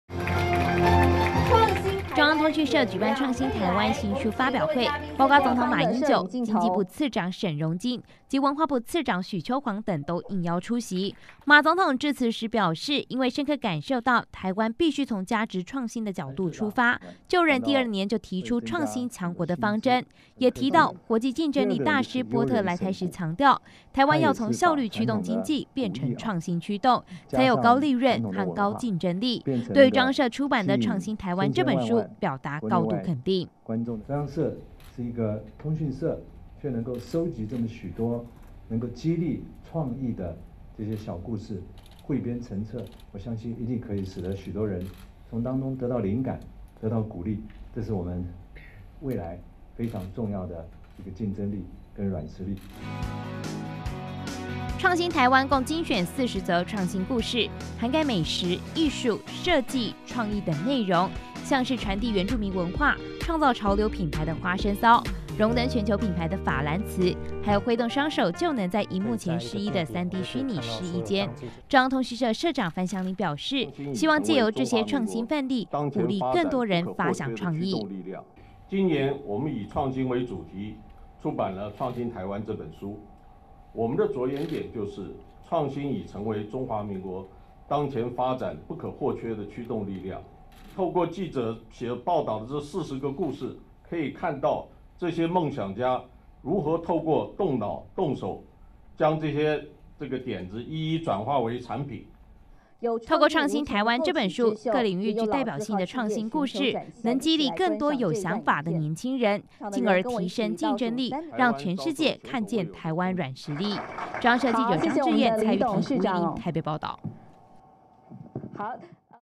創新台灣新書發表 總統出席
中央通訊社舉辦創新臺灣新書發表會，包括總統馬英九、經濟部次長沈榮津及文化部次長許秋煌等都應邀出席。馬總統致詞時表示，因為深刻感受到臺灣必須從加值創新的角度出發，就任第二年就提出創新強國的方針，也提到國際競爭力大師波特來臺時強調，臺灣要從效率驅動經濟變成創新驅動，才有高利潤和高競爭力，對於中央社出版的創新臺灣這本書，表達高度肯定。